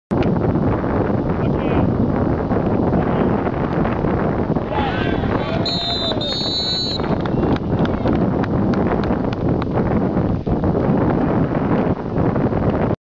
2008.09.28 Sęp Żelechów (juniorzy) - Żak Szreńsk: bramka Seweryna Staniszewskiego
Druga bramka w tym meczu dla Sępa w wykonaniu Seweryna Staniszewskiego.